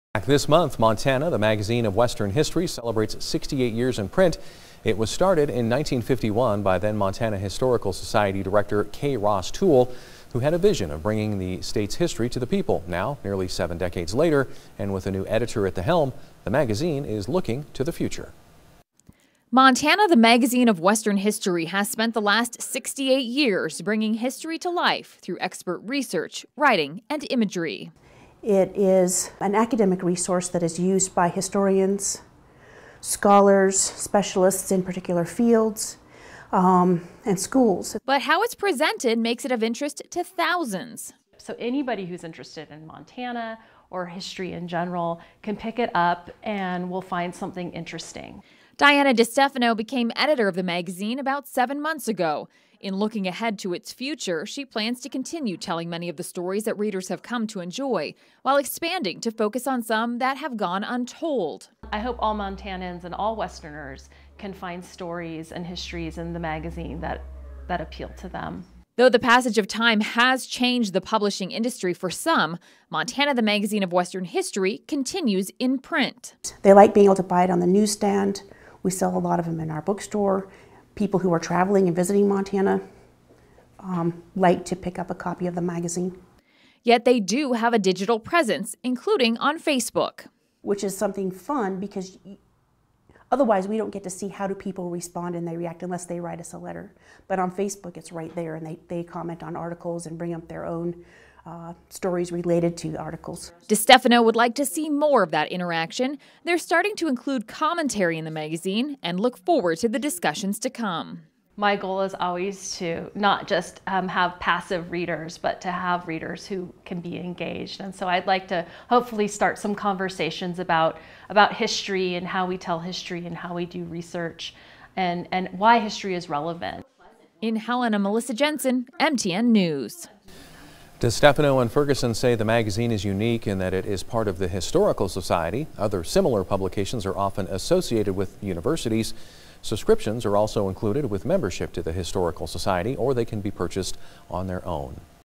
Montana The Magazine of Western History celebrates 68 years
The publication was started in 1951 by a man who had a vision of bringing the state’s history to the people.